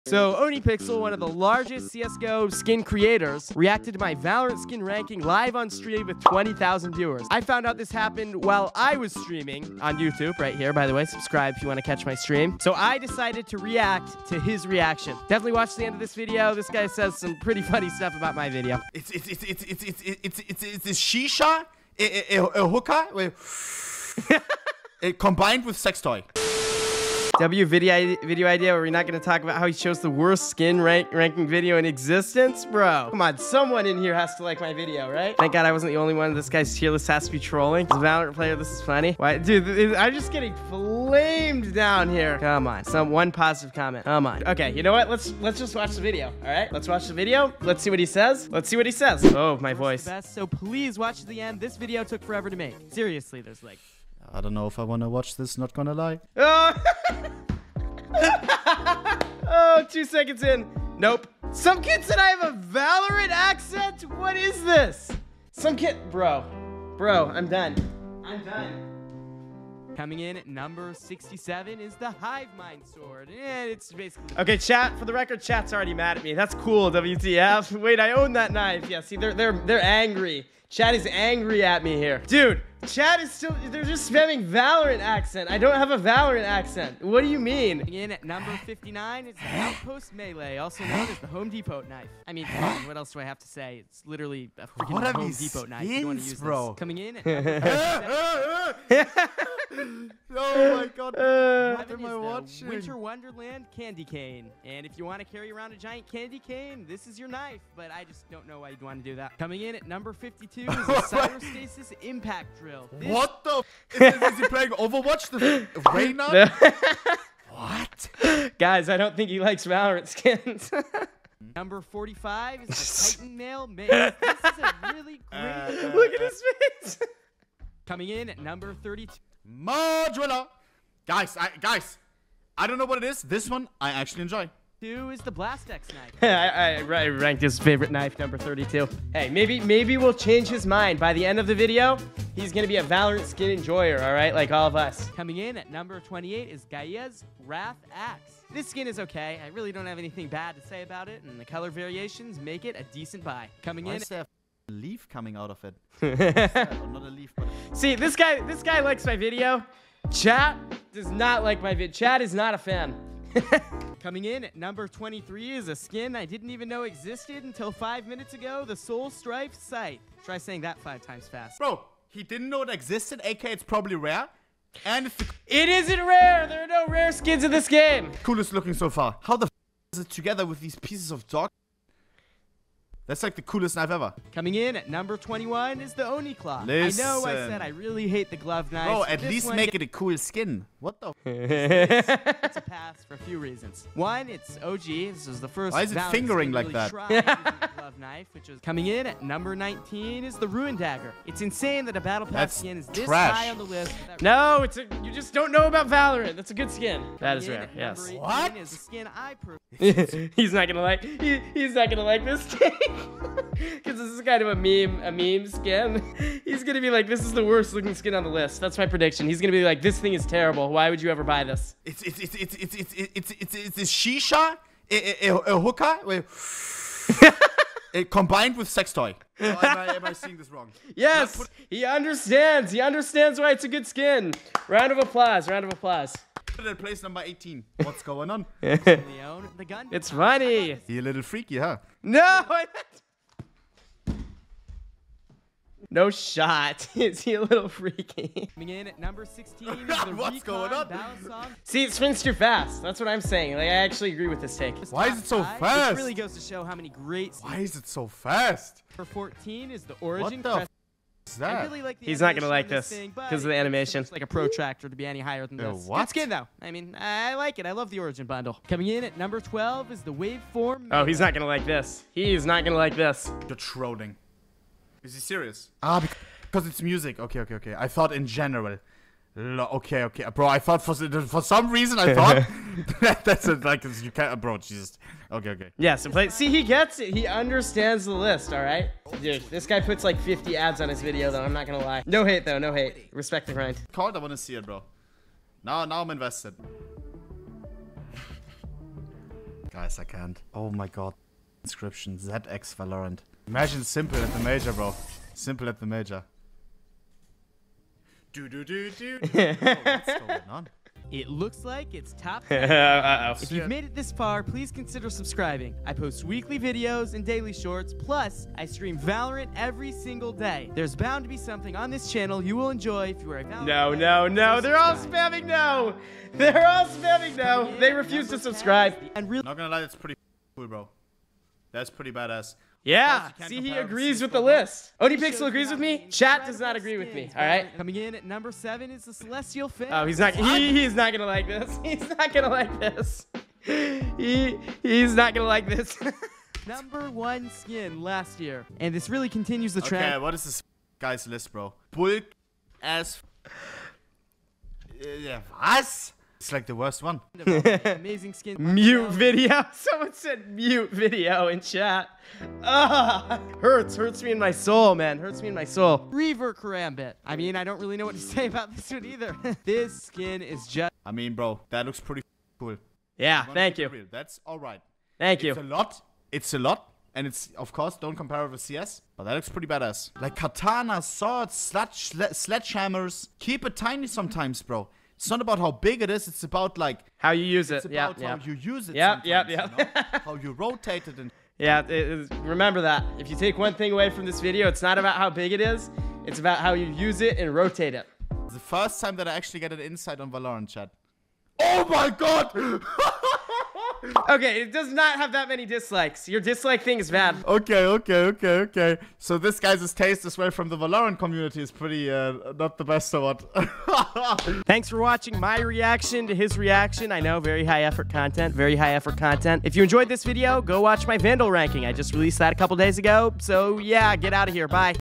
CSGO's LARGEST Streamer Reacted to my Video (ohnePixel)
This was a surreal experience watching this many people react to my video. I think he liked it? Thank you ohnePixel for the reaction!